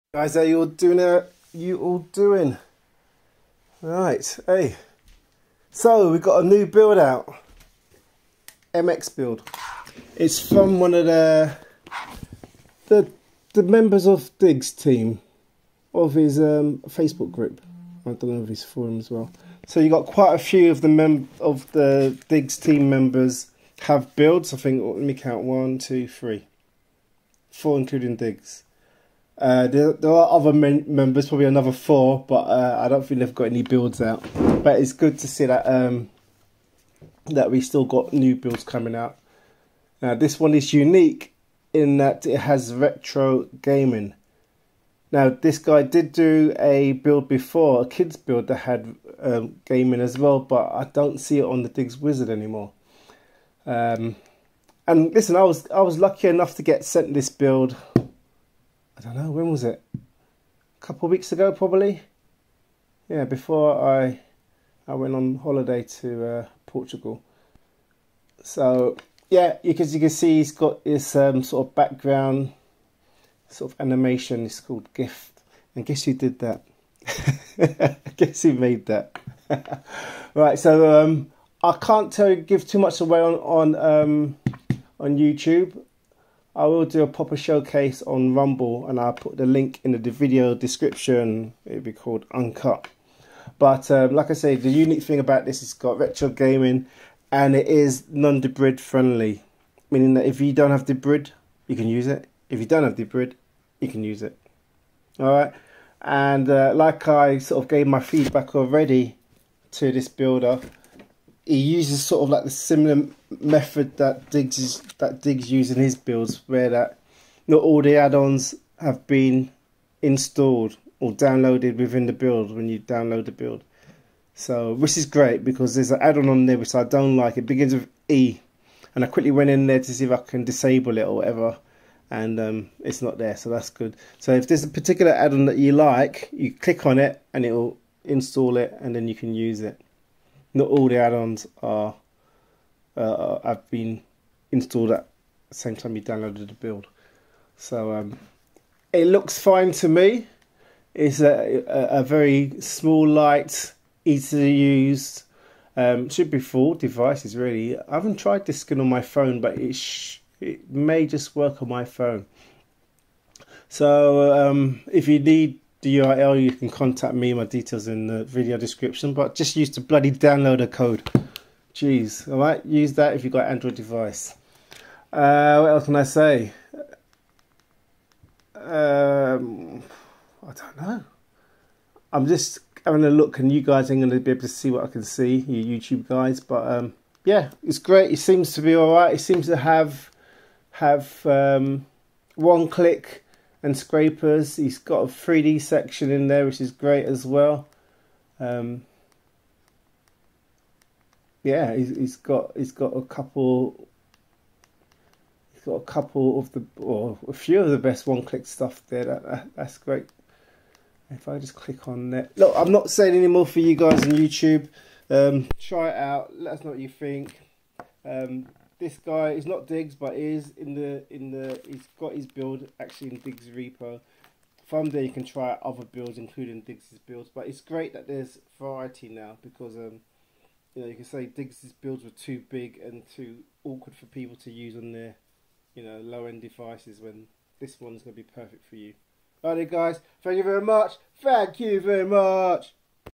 MX Build - FREE & Debrid Easy to use Kodi Build From the Diggz Team - Install Kodi Fork
Uncut

You may need to relaunch the build a few times.

Zippo’s Media
Forks 867976

Setting used on Builds for Dolby Atmos, DTSX and Dolby True HD